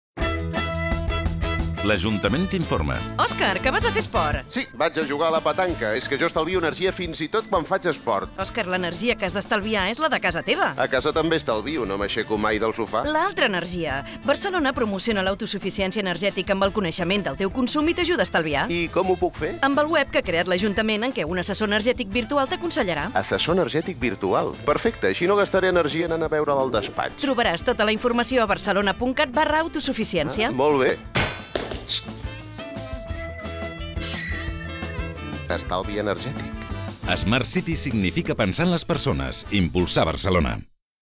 L'Ajuntament informa: Smart City - Estalvi energètic
Vols saber com pots estalviar energia en funció del teu consum? Al web d'autosuficiència energètica de l'Ajuntament de Barcelona un assessor virtual t'aconsellarà de manera personalitzada com reduir el consum a la teva llar.